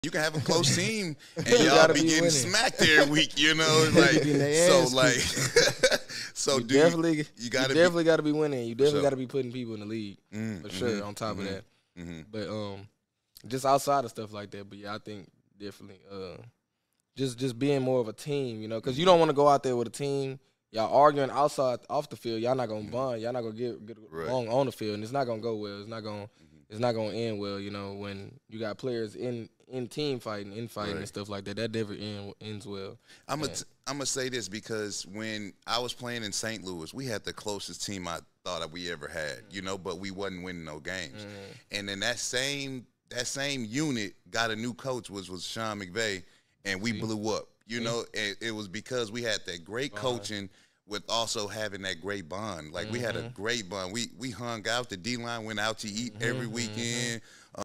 Winning Starts with Team Bonding and Trust | Michael Brockers & Maurice Williams II
Michael Brockers & Maurice Williams II touch on why good team bonding is an important piece for success.

Produced and Edited by:
Julius Williams

Videographer/Editor:
Troy

Social Media Manager:
Dasia Batiste